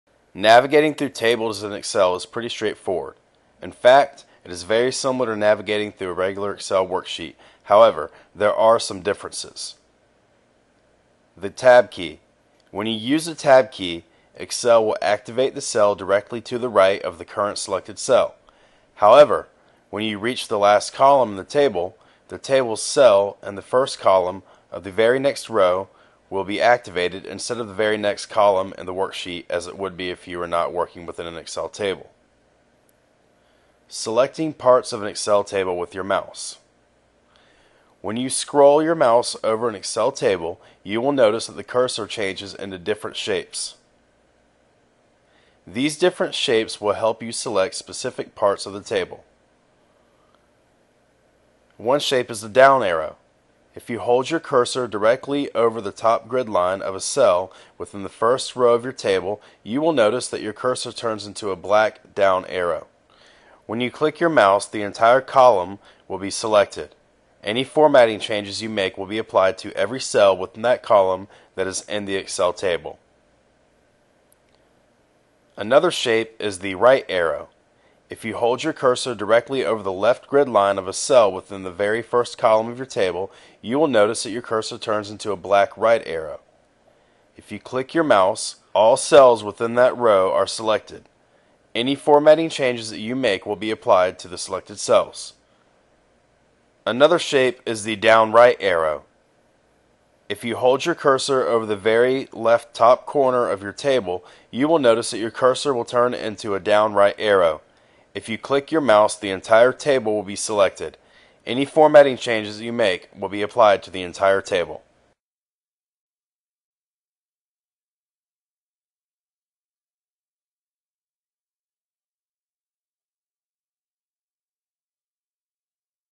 Excel 2013 Tutorial For Noobs Part 34: How to Navigate Through Tables in Excel
In this Microsoft Excel Tutorial for beginners I show you how to navigate through table in Excel. In this video tutorial I am using Microsoft Excel 2013 but this tutorial also applies to Microsoft Excel 2010 and Microsoft Excel 2007.